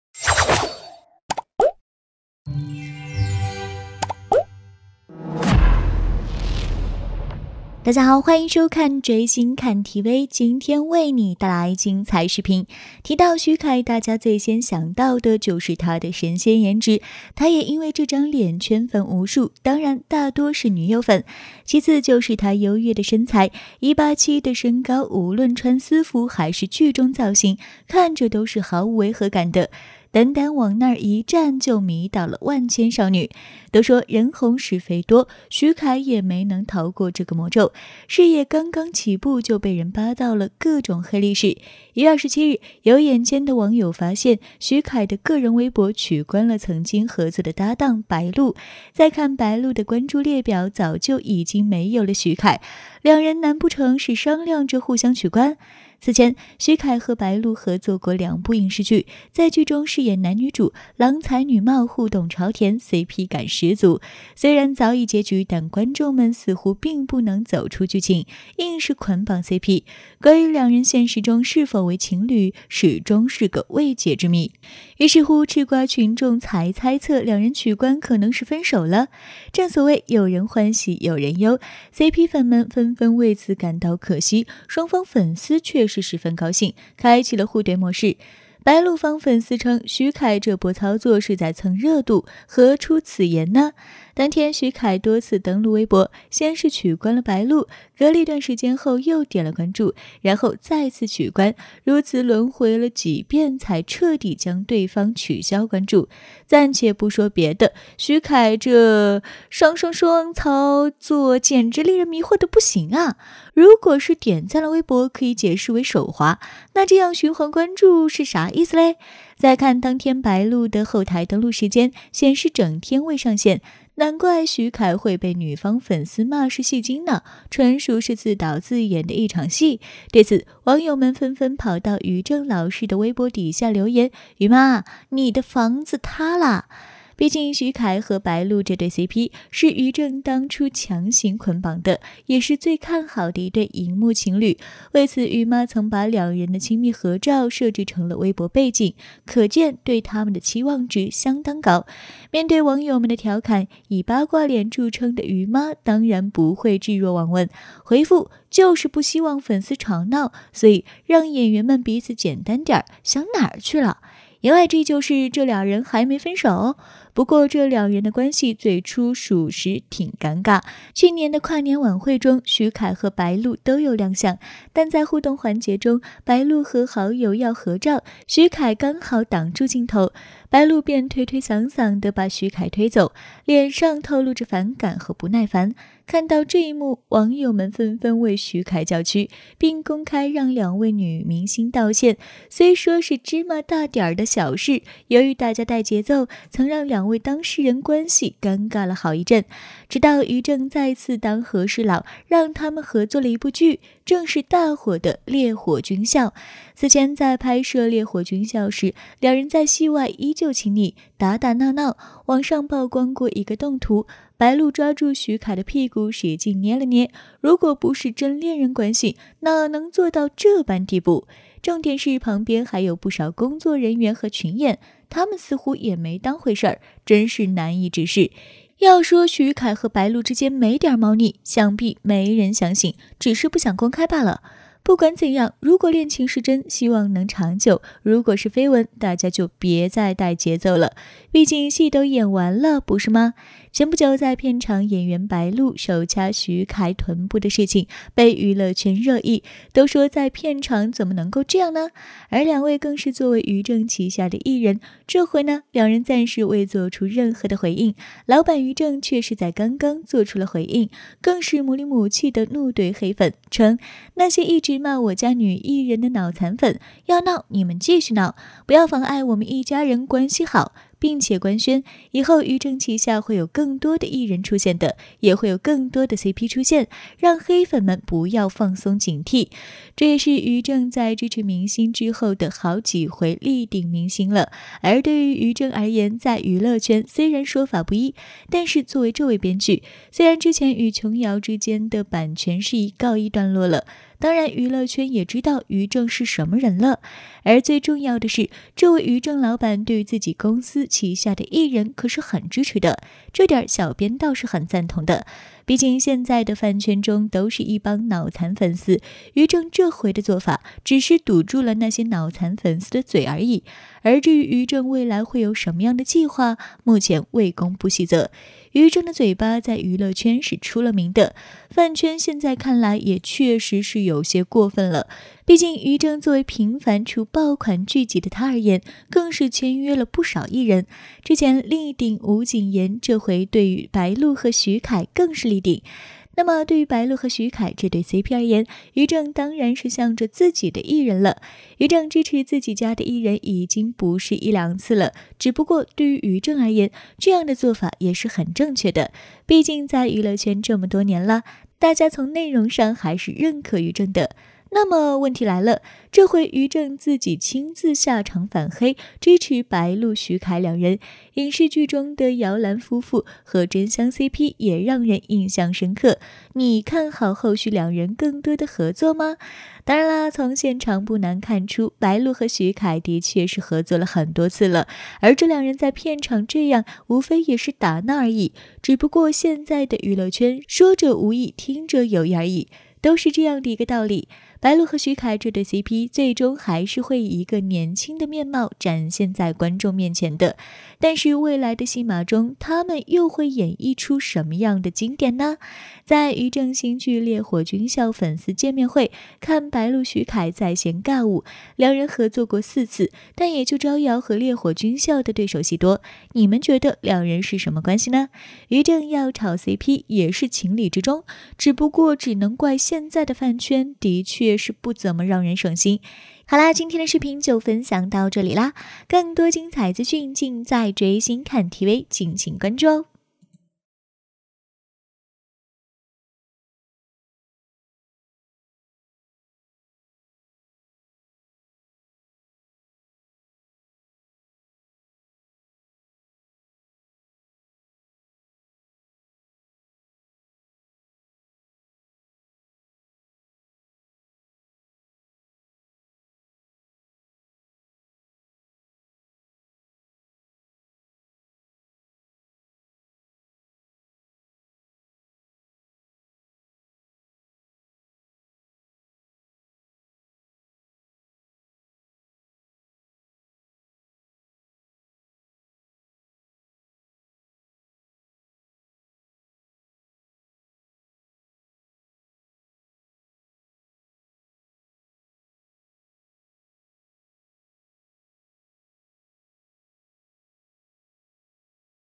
許凱白鹿官宣結婚！同居100張親密照流出實錘！又一對緋聞cp開花結果惹人羨！靜待官宣！原來任嘉倫只是擋箭牌！真愛心有所屬！追星看TV
許凱白鹿官宣結婚！同居100張親密照流出實錘！又一對緋聞cp開花結果惹人羨！靜待官宣！原來任嘉倫只是擋箭牌！真愛心有所屬！追星看TV#許凱#白鹿